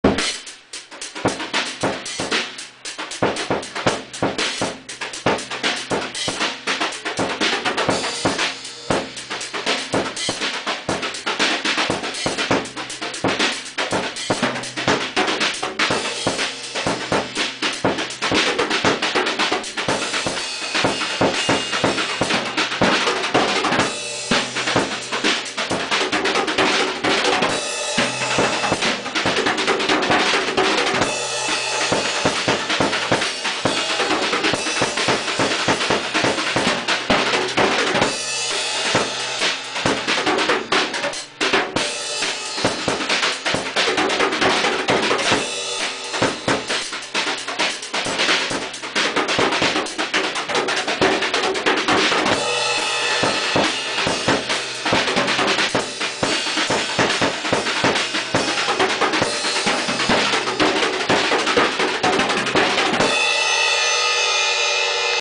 Joshua playing the drum set
Joshua BartonLeingang playing drums in the shed